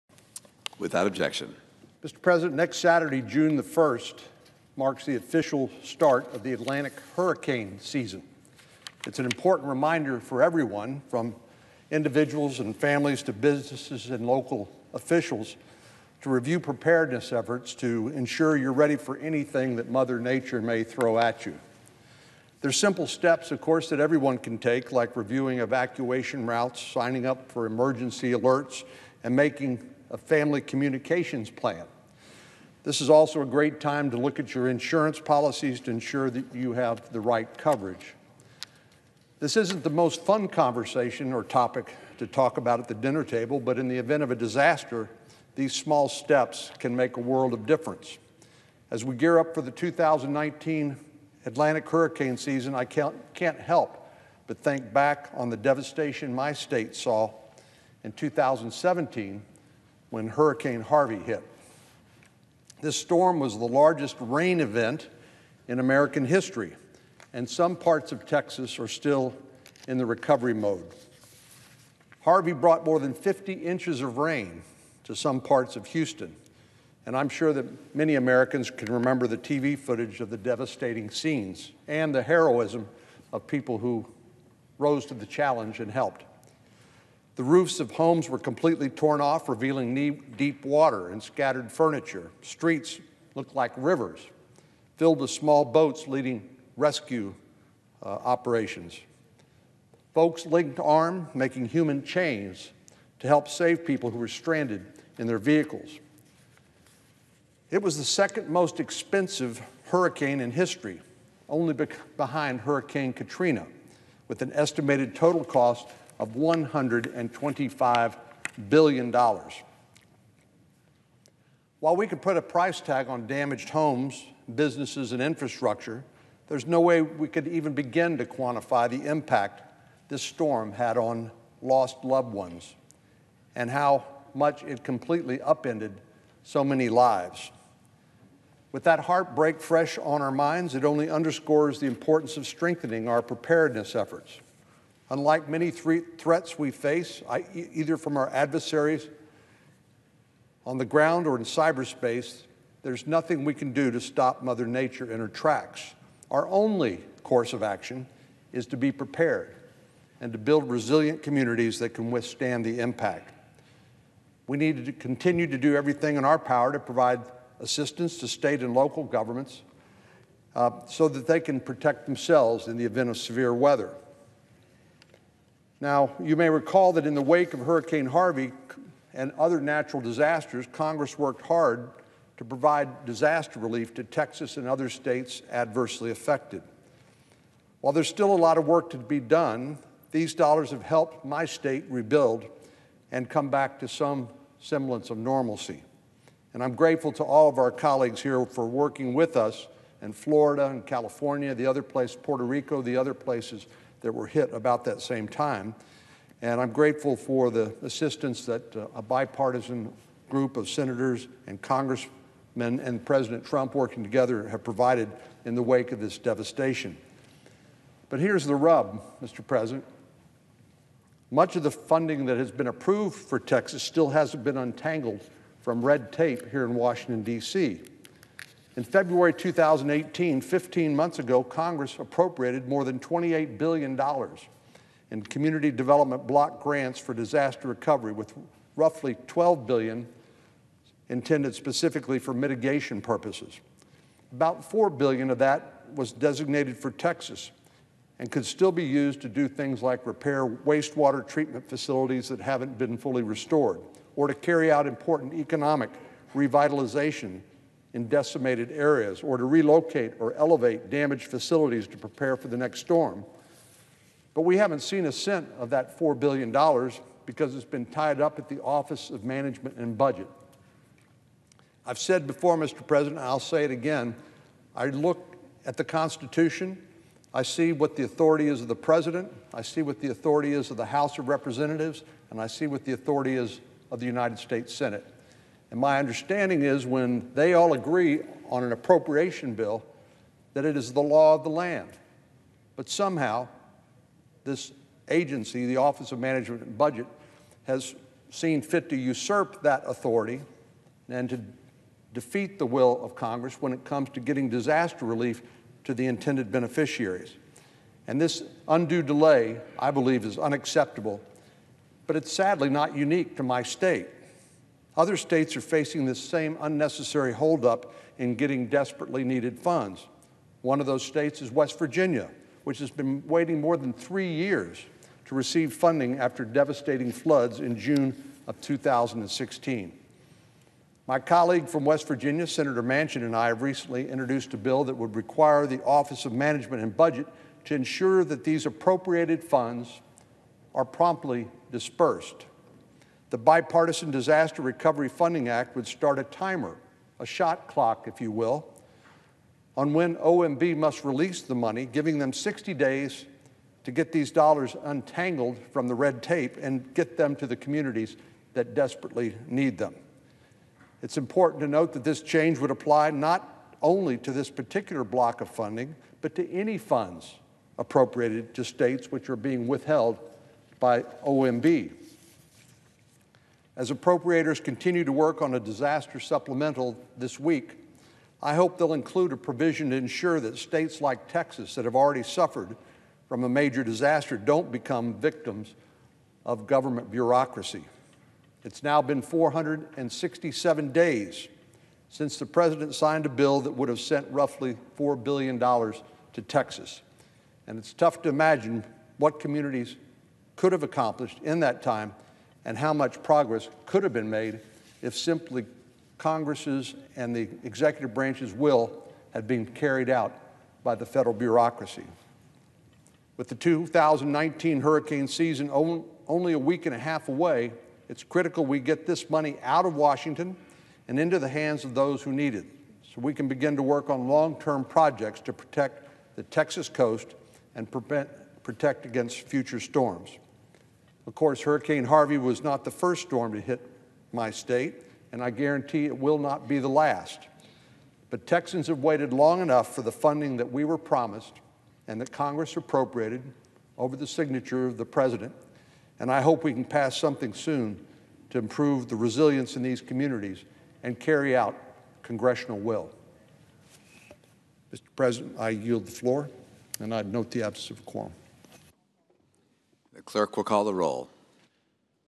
Senator John Cornyn Again Urges OMB to Release $4B in Harvey Relief as Hurricane Season Approaches
Today on the floor, U.S. Senator John Cornyn (R-TX) discussed the Office of Management and Budget’s delay in releasing much-needed Hurricane Harvey recovery funds ahead of this year’s hurricane season. He also encouraged his colleagues to include his bill to speed up the funding’s release in any disaster supplemental funding agreement this week.